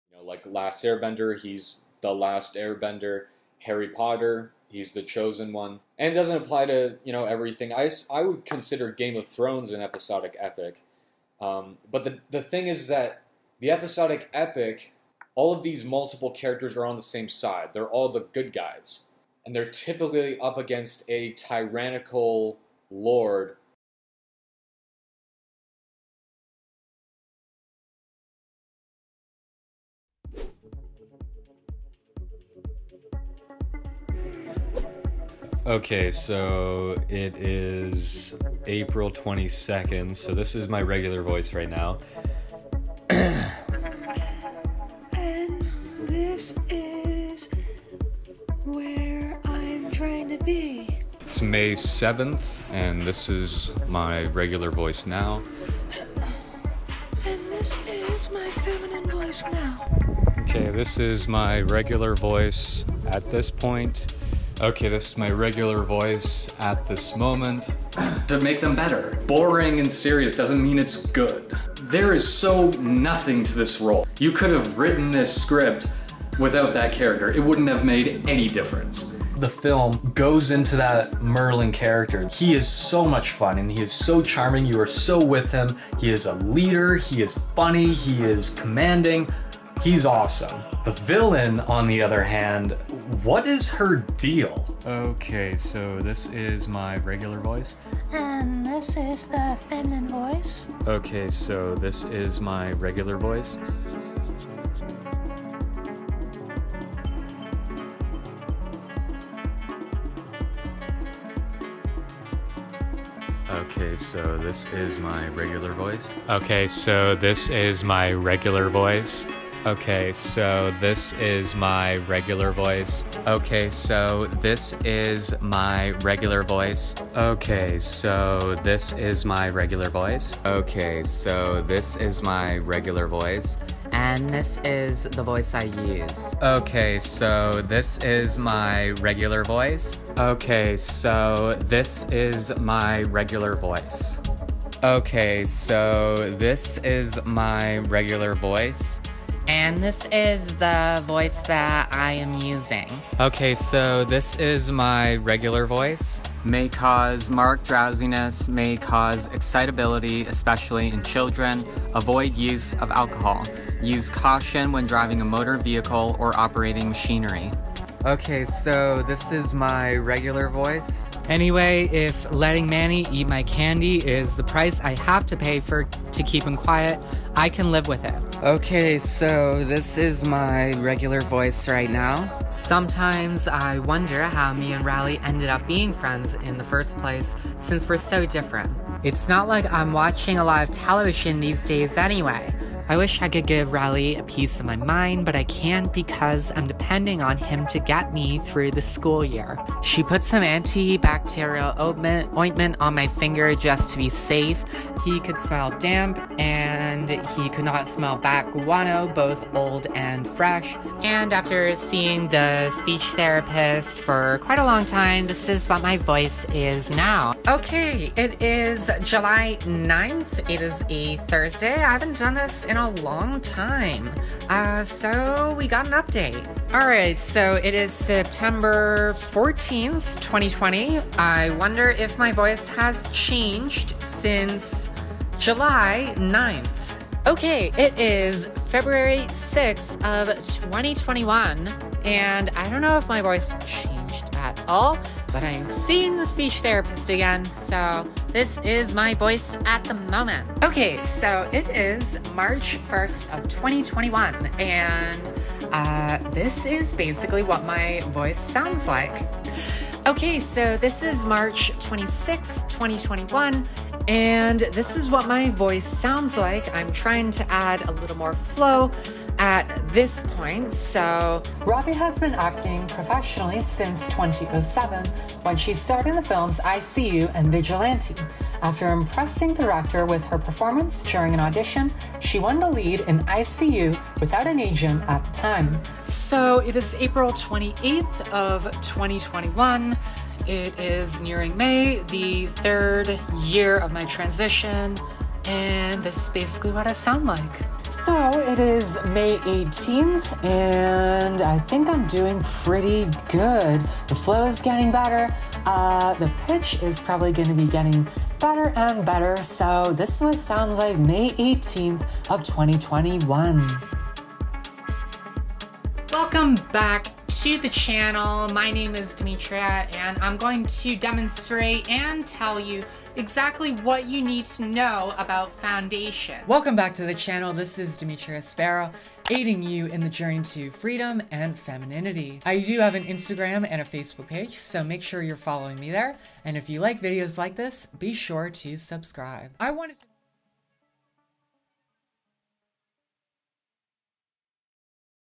MTF Voice Transition (3-Year Timeline) | DIMITRIA SPARROW
Thought I had some extra audio and footage to make this its own video. A voice feminization journey over the course of 3+ years! Hope this video proves to you that anything's possible in this lifetime!